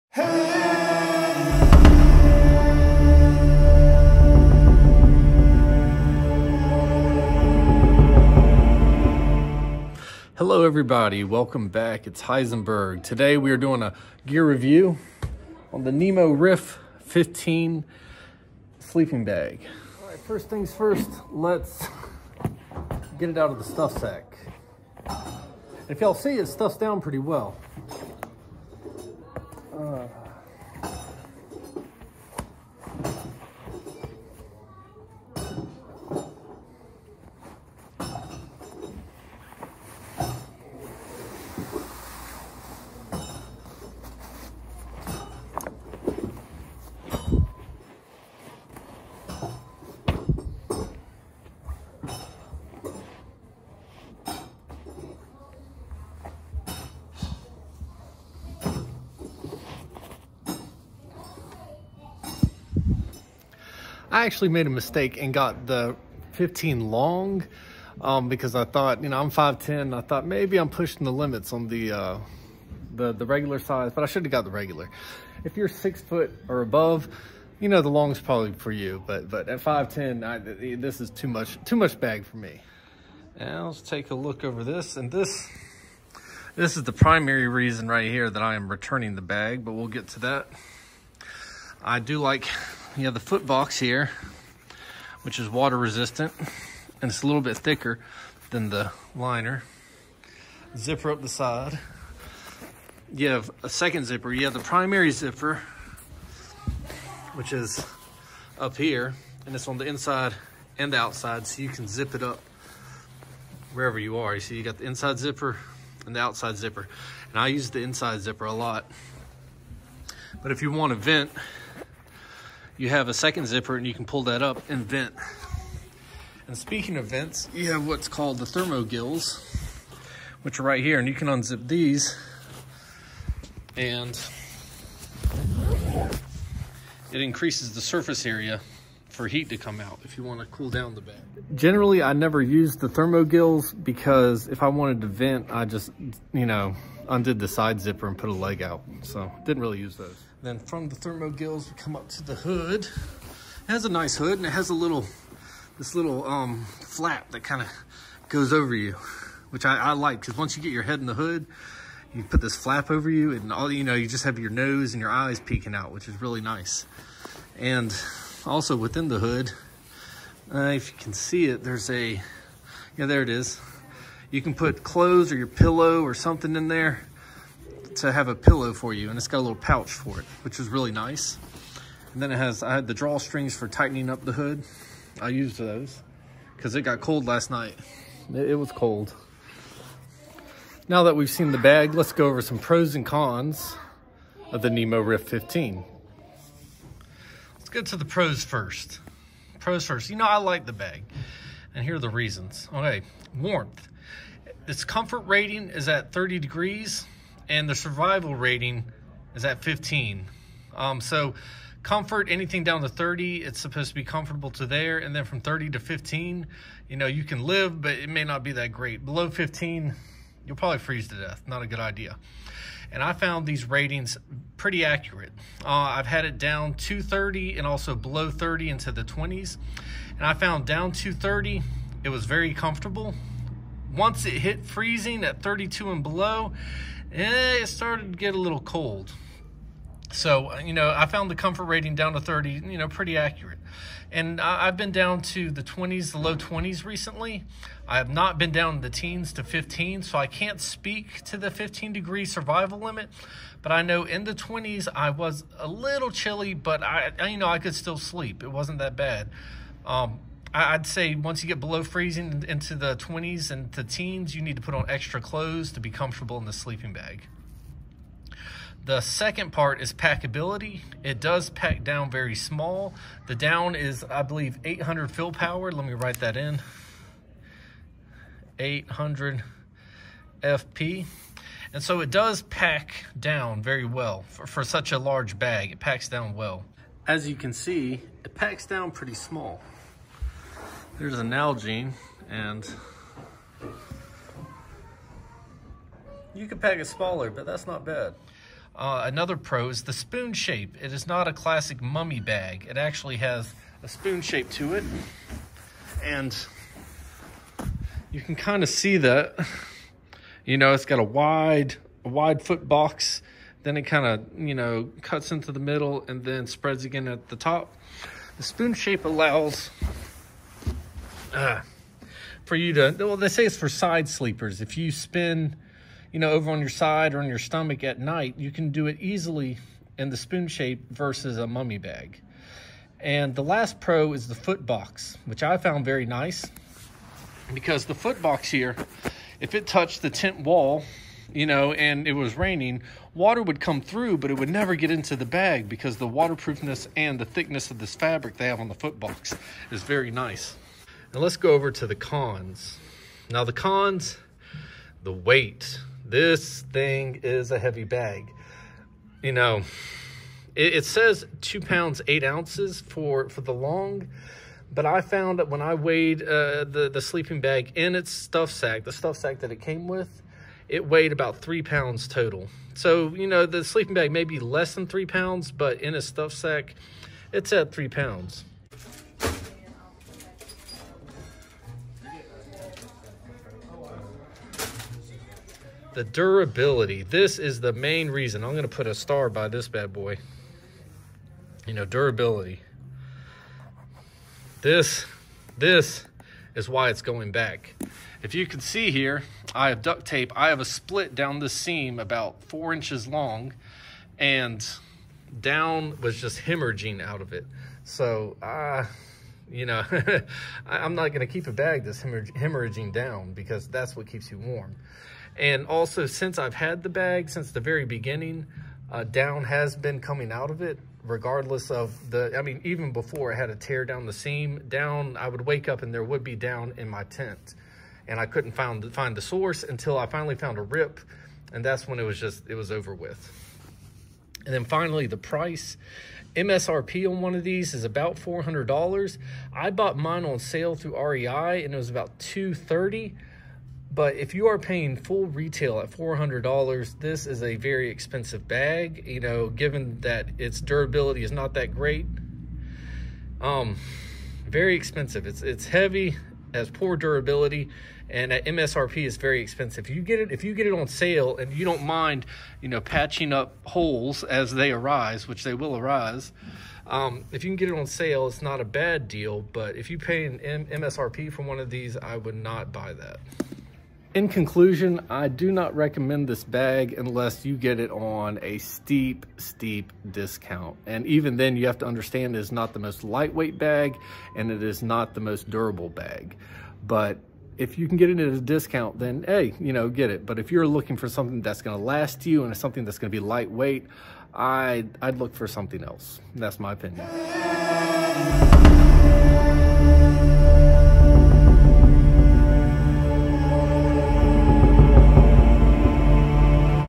Nemo Riff 15 Sleeping Bag Review
Let’s check out the pros and cons of the Nemo Riff 15.